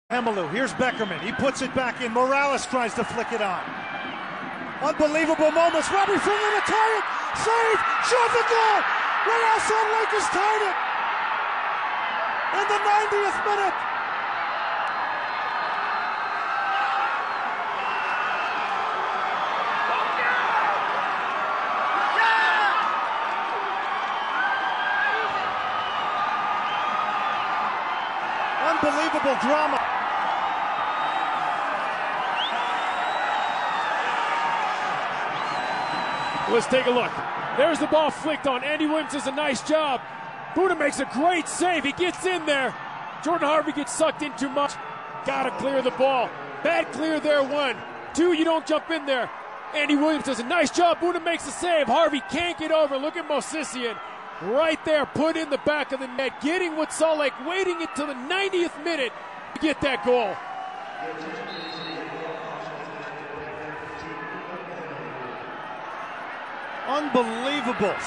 Real Salt Lake - Headed to the Playoffs!
Yura Movsisyan's 90th minute goal sends RSL to the playoffs for the first team in club history!